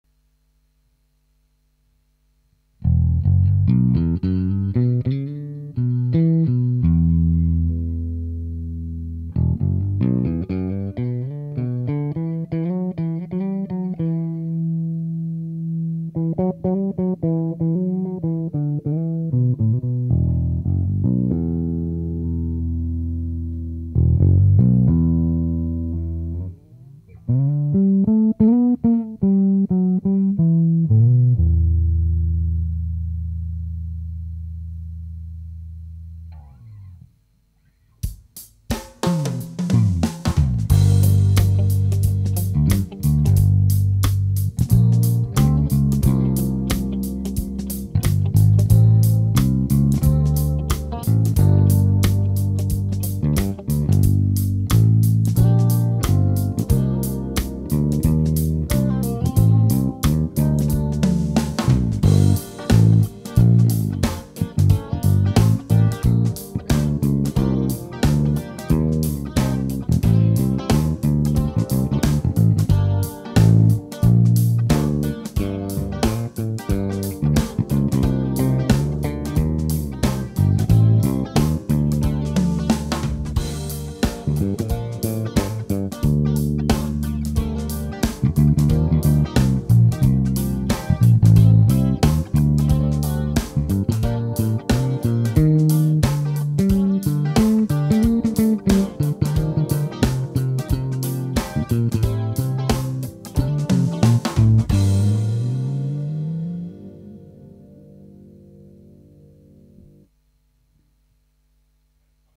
Fender Jazz Bass from 1969 presented by Vintage Guitar and Detlef Blanke
Vintage Guitar Oldenburg and Detlef Blanke present a Fender Precision bass from 1969.
It was played over a realbass Duo amp with 2x10" cab. You hear on the left side the DI-direct sound and on the right the sound miked with a Sennheiser E-602 microphone.